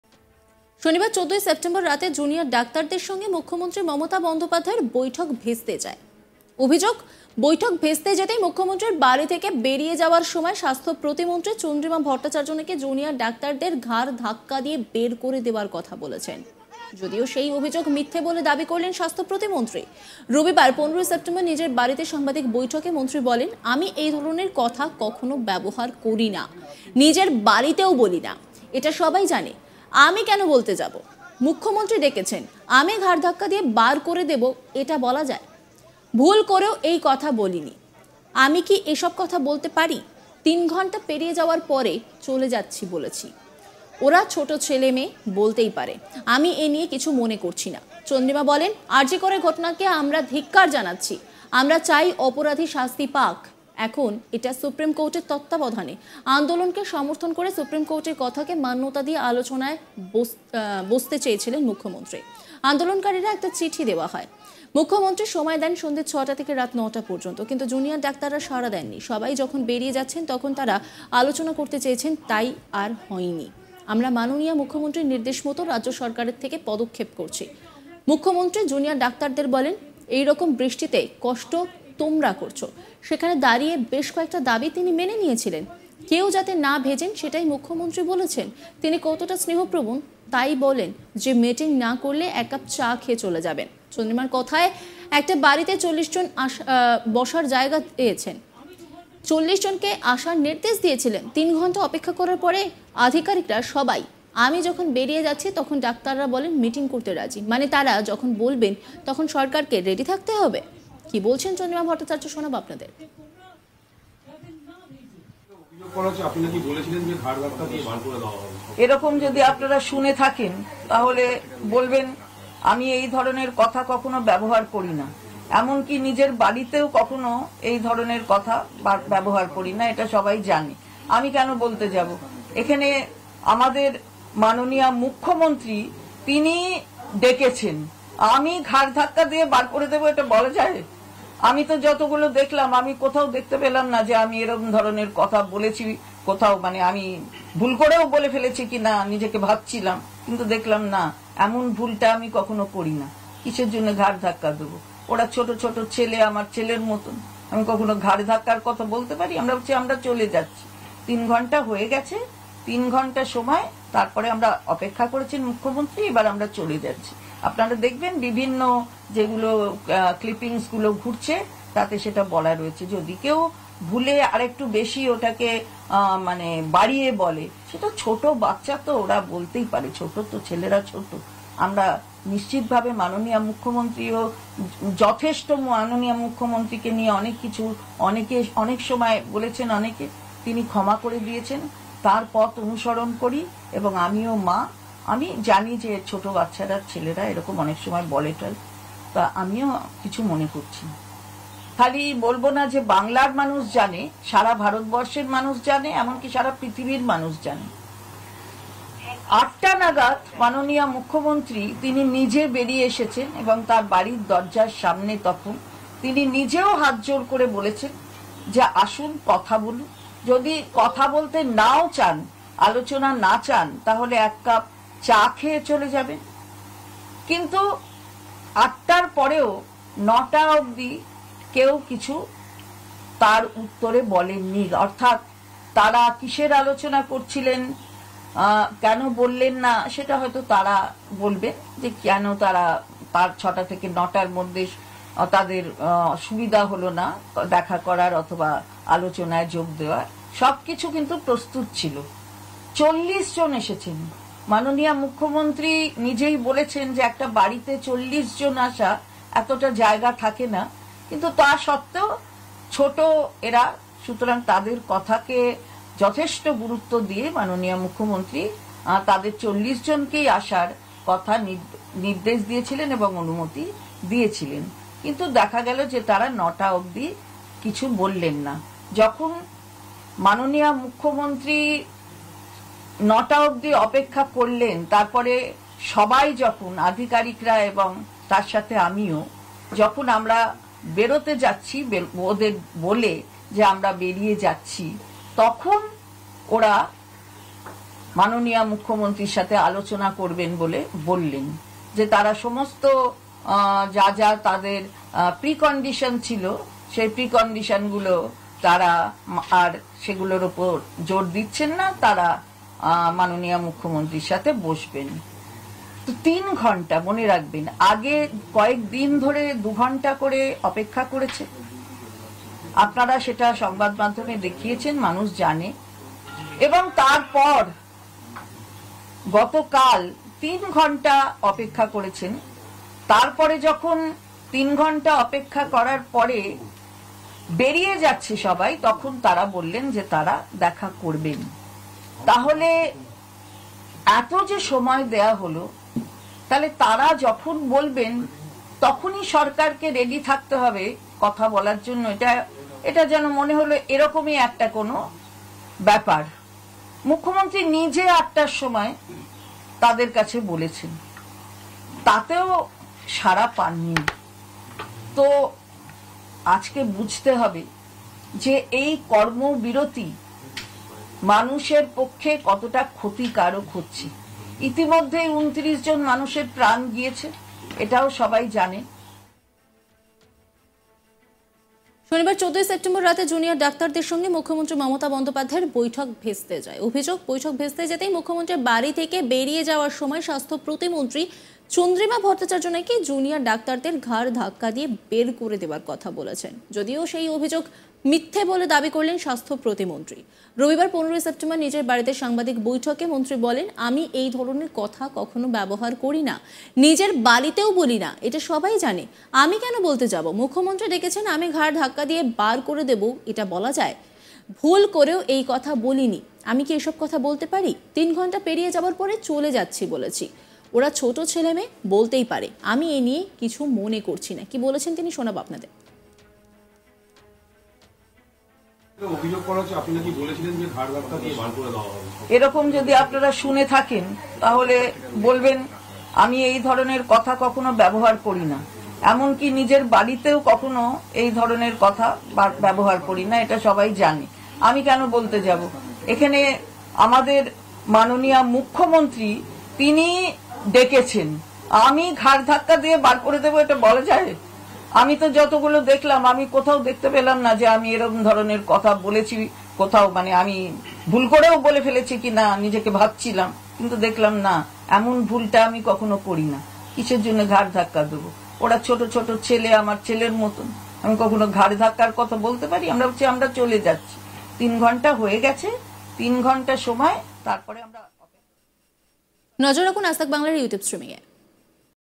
'ওরা আমার ছেলের মতো, ঘাড়ধাক্কার কথা বলিনি,' চন্দ্রিমার বড় সাফাই শুনুন | Chandrima |RG Kar Protest
শনিবার, 14 সেপ্টেম্বর রাতে জুনিয়র ডাক্তারদের সঙ্গে মুখ্যমন্ত্রী মমতা বন্দ্যোপাধ্যায়ের বৈঠক ভেস্তে যায়। অভিযোগ, বৈঠক ভেস্তে যেতেই মুখ্যমন্ত্রীর বাড়ি থেকে বেরিয়ে যাওয়ার সময় স্বাস্থ্য প্রতিমন্ত্রী চন্দ্রিমা ভট্টাচার্য নাকি জুনিয়র ডাক্তারদের ঘাড়ধাক্কা দিয়ে বের করে দেওয়ার কথা বলেছেন। যদিও সেই অভিযোগ মিথ্য়া বলে দাবি করলেন স্বাস্থ্য প্রতিমন্ত্রী। রবিবার, ১৫ সেপ্টেম্বর নিজের বাড়িতে সাংবাদিক বৈঠকে মন্ত্রী বলেন, 'আমি এই ধরনের কথা কখনও ব্যবহার করি না। নিজের বাড়িতেও বলি না। এটা সবাই জানে। আমি কেন বলতে যাব। মুখ্যমন্ত্রী ডেকেছেন, আমি ঘাড়ধাক্কা দিয়ে বার করে দেব এটা বলা যায়। ভুল করেও এই কথা বলিনি। আমি কি এসব কথা বলতে পারি। তিন ঘণ্টা পেরিয়ে যাওয়ার পরে চলে যাচ্ছি বলেছি। ওরা ছোট ছেলে-মেয়ে, বলতেই পারে। আমি এনিয়ে কিছু মনে করছি না।' চন্দ্রিমা বলেন, 'আরজি করের ঘটনাকে আমরা ধিক্কার জানাচ্ছি। আমরা চাই অপরাধী শাস্তি পাক। এখন এটা সুপ্রিম কোর্টের তত্ত্বাবধানে। আন্দোলনকে সমর্থন করে সুপ্রিম কোর্টের কথাকে মান্যতা দিয়ে আলোচনায় বসতে চেয়েছিলেন মুখ্যমন্ত্রী। আন্দোলনকারীরা একটা চিঠি দেওয়া হয়। মুখ্যমন্ত্রী সময় দেন সন্ধে ৬টা থেকে রাত ৯টা পর্যন্ত । কিন্ত জুনিয়র ডাক্তাররা সাড়া দেননি। সবাই যখন বেরিয়ে যাচ্ছেন তখন তাঁরা আলোচনা করতে চেয়েছেন, তাই আর হয়নি। আমরা মাননীয়া মুখ্যমন্ত্রীর নির্দেশ মতো রাজ্য সরকারের থেকে পদক্ষেপ করছি। মুখ্যমন্ত্রী জুনিয়র ডাক্তারদের বলেন, এইরকম বৃষ্টিতে কষ্ট তোমরা করছ, সেখানে দাঁড়িয়ে বেশ কয়েকটা দাবি তিনি মেনে নিয়েছিলেন। কেউ যাতে না ভেজেন সেটাই মুখ্যমন্ত্রী বলেছেন। তিনি কতটা স্নেহপ্রবণ, তাই বলেন যে  মিটিং না করলে এক কাপ চা খেয়ে চলে যাবেন।' চন্দ্রিমার কথায়, 'একটা বাড়িতে ৪০ জন আসা, জায়গা দিয়েছেন। ৪০ জনকেই আসার নির্দেশ দিয়েছিলেন। তিন ঘণ্টা অপেক্ষা করার পরে আধিকারিকরা সবাই, আমি যখন বেরিয়ে যাচ্ছি,তখন ডাক্তাররা বলেন মিটিং করতে রাজি। মানে তাঁরা যখন বলবেন, তখনই সরকারকে রেডি থাকতে হবে?'